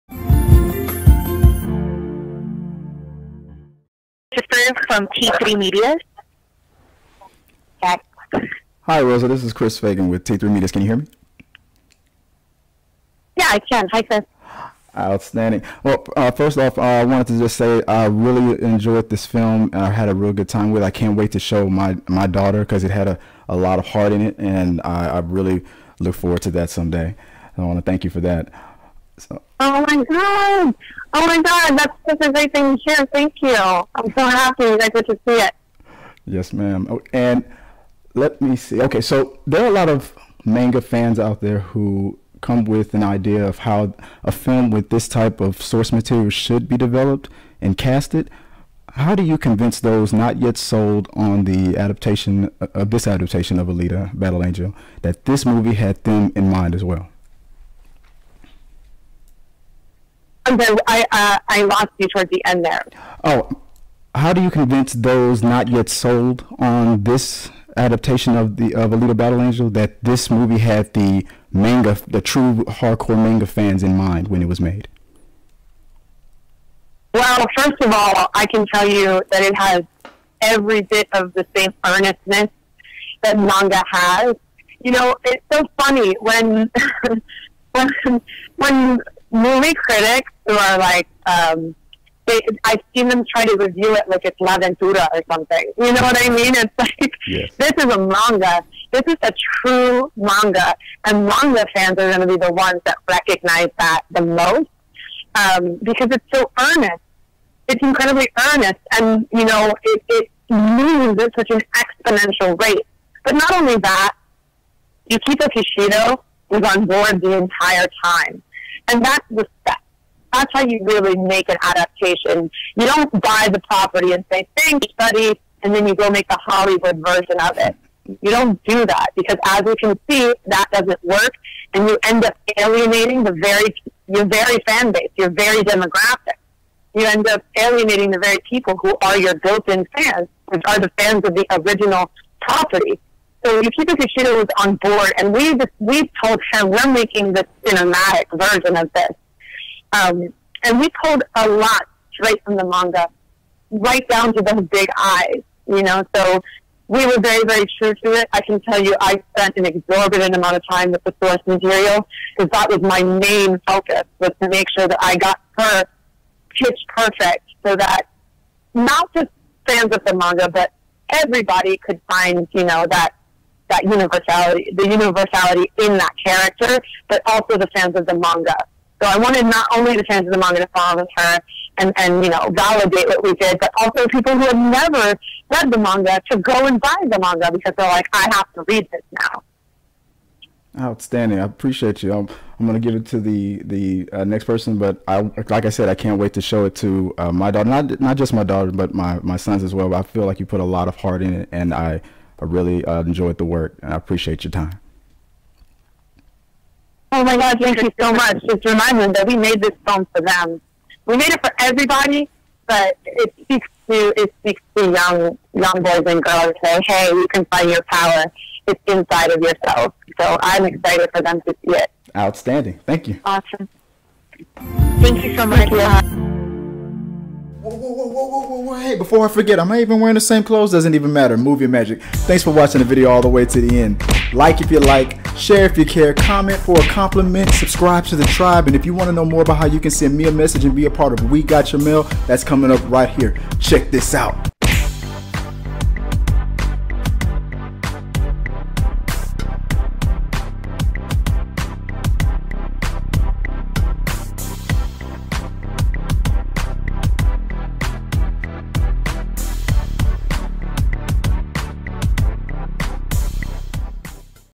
OneOnOne with Rosa Salazar Interview
Video Description:
And now we're going to go with Christopher with T3Medias

Chris: hi Rosa this is Chris Fagan with T3Medias.

Rosa : Hi Chris

Chris: Outstanding. Well first off I wanted to say I really enjoyed this film I had a real good time and I can't wait to show my daughter because it had a lot of heart in it. And I really look forward to that someday and I want to thank you for that.

Rosa: oh my God! oh my God that's such a great thing to hear! thank you I'm so happy that they get to see it.

Chris: yes ma'am Okay so there are a lot of manga fans out there that come with an idea of how I film with this type of source material should be developed and cast it. How did you convince those not yet sold on the adaptation of Alita battle angel that this movie had them in mind as well?

Rosa: I lost you at the in there

Chris: how do you convince those not yet sold on this adaptation of Alita battle angel that this movie had the manga the true manga fans in mind when it was made.

Rosa: well first of all I can tell you that this movie has all of the same bits of earnestness that the manga has. You know it's so funny. she laughs. Movie critics I see them try to review it like it's La Duda or something. You know what I mean? This is Amanda, this is a true manga and manga fans are going to be the ones to recognize that the most because it's so Earnest. It's incredibly Earnest. And you know it's moves in such an exponential rate. But I don't like that. Yukito cash it or was I'm bored the entire time. That's how you really make an adaptation. You don't buy the priority and just say thanks buddy and then make a Hollywood version of it. as you can see that doesn't work and you end up alienating you're very fanbase and demographic and the Very people who are the fans of the original property. and we are making a cinematic version of this and we put a lot straight from the manga right down to those big eyes. So we are very very true to it and I can tell you I spent an exorbitant amount of time with it making it my main focus making sure she was Pitch Perfect. Not just for fans of the manga but everybody can find the universality of the character but also the fans of the manga. So also the fans of the manga could validate and people who never read the Manga could go by the manga and say I have to go read this now.

Chris: Outstanding I appreciate you. but like I said I can't wait to show it to not just my daughter but my sons as well I feel like you put a lot of heart in it and I really enjoyed the work and I appreciate your time.

Rosa: oh my God thank you so much and you just remind them that we made this film for them. We made it for everybody it speaks to young boys and girls and say Hey you can find your power it's inside of yourself so I'm very excited for them to see it.

Chris: Outstanding thank you.

Thank you to Rosa Salazar
Set several centuries in the future, the abandoned Alita is found in the scrapyard of Iron City by Ido, a compassionate cyber-doctor who takes the unconscious cyborg Alita to his clinic. When Alita awakens, she has no memory of who she is, nor does she have any recognition of the world she finds herself in. As Alita learns to navigate her new life and the treacherous streets of Iron City, Ido tries to shield her from her mysterious past.
Release date: February 14, 2019 (USA)
Director: Robert Rodriguez
Budget: 200 million USD
Producers: James Cameron, Robert Rodriguez, Jon Landau
Production companies: 20th Century Fox, Lightstorm Entertainment, Troublemaker Studios

Talking Through The Medias Live every Wednesday
New videos post Thursday & Saturday